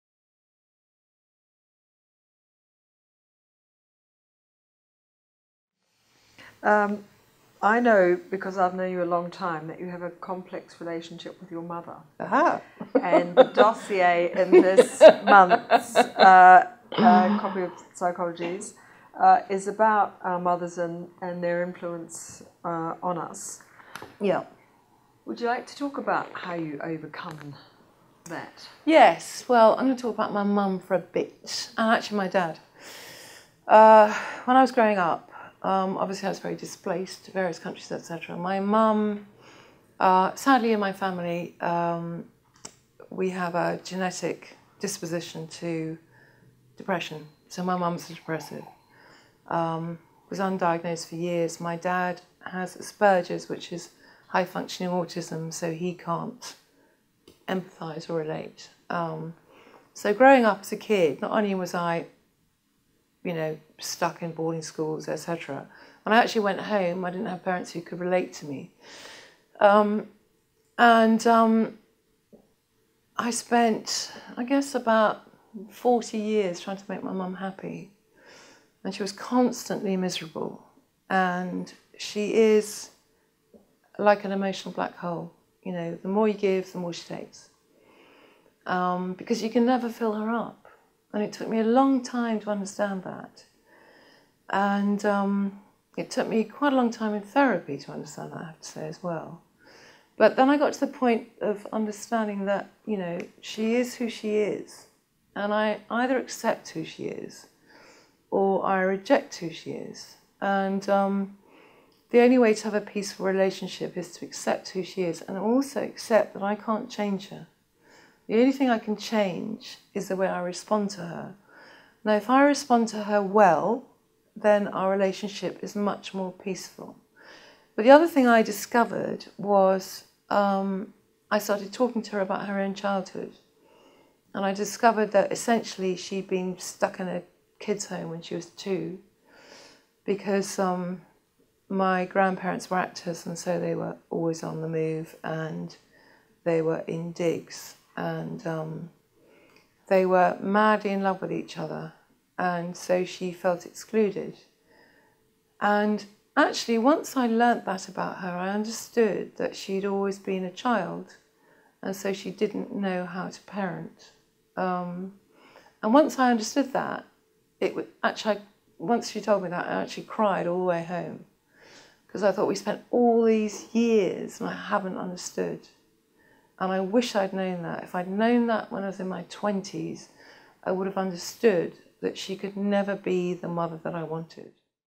Sally Brampton on her relationship with her mother
At our Psychologies reader event, our editor Louise Chunn talked to our columnist Sally Brampton about depression, being perfect and how our mothers shape us.